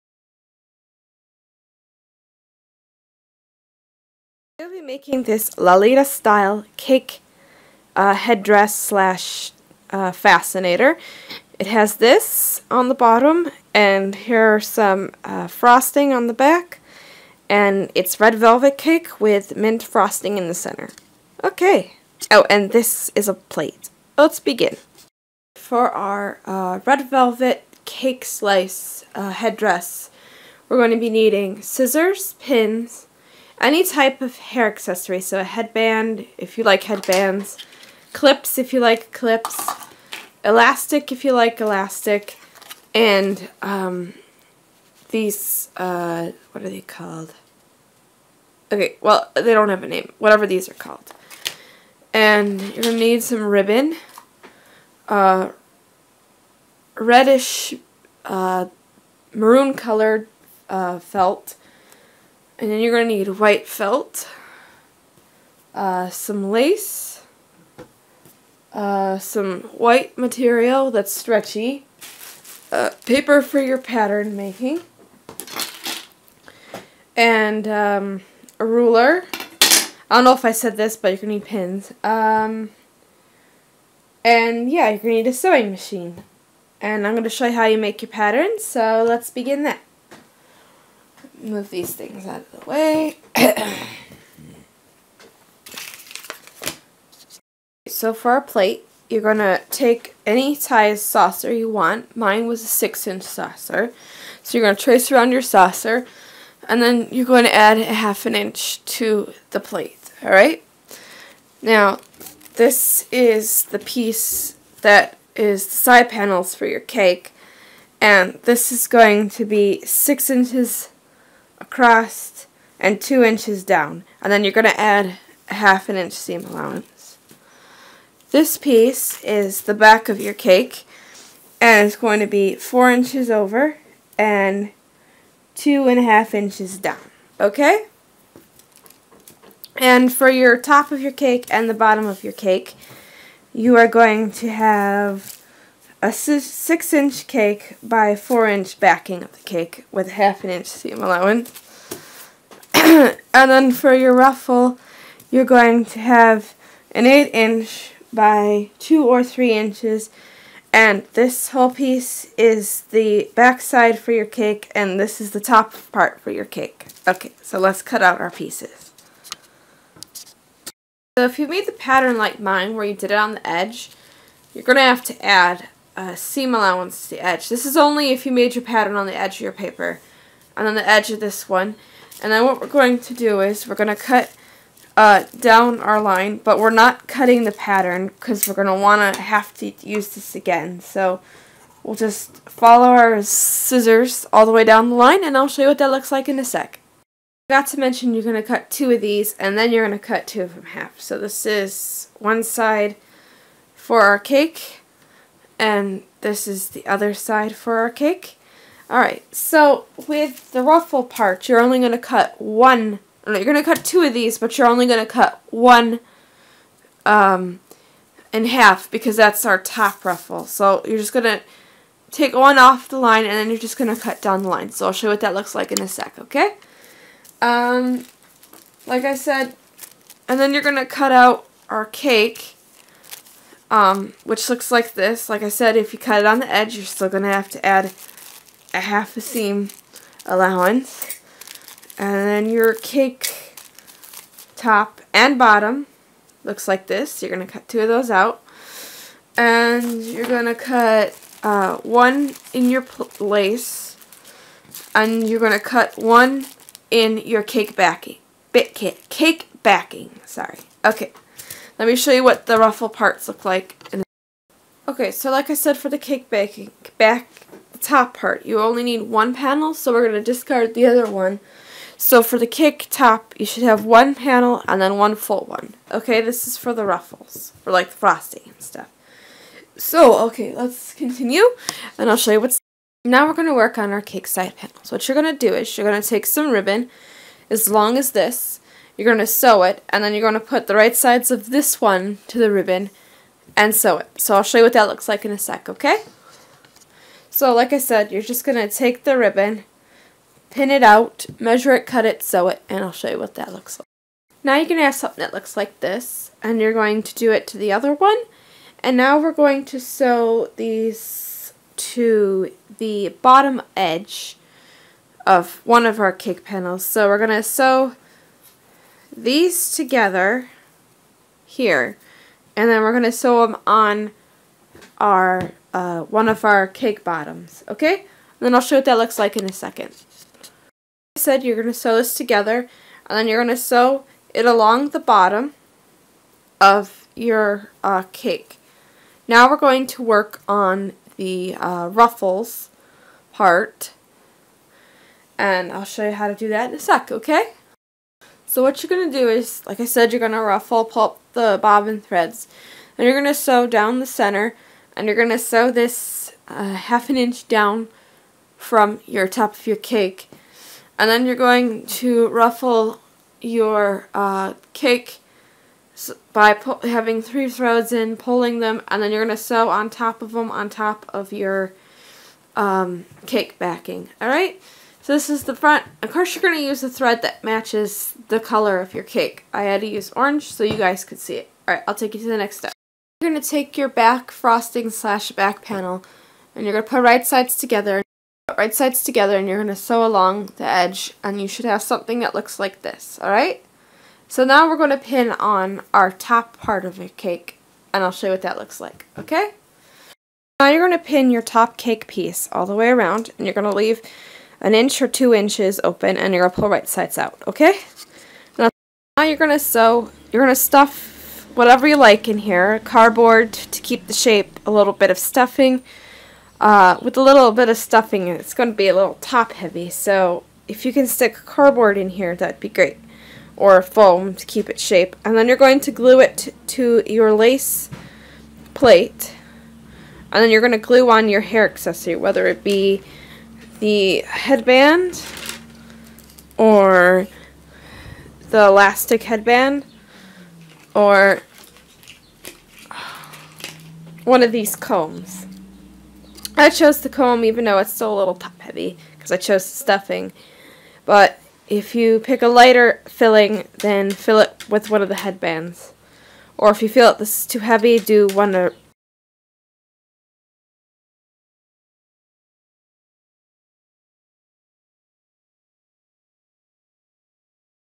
how to sew a lolita cake headdress or anything you like
You will need felt any color will do ribbon or basis tape in any color you want lace pattern scissors any kind of fabric for top and for the ruffles head band iron ironing board stuffing hot glue